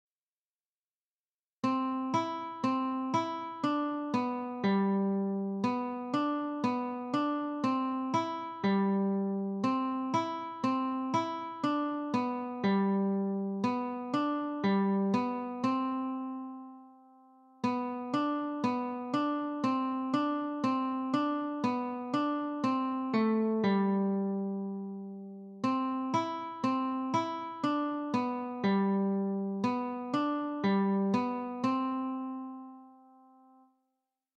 Three String Study - Beginner Guitarist - Play-Along Music & Tab
Simple three-string study for the beginner guitarist.  Remember to anticipate notes on different strings with the right hand using all of your fingers including the thumb on the G string.  Aim for all notes to ring out.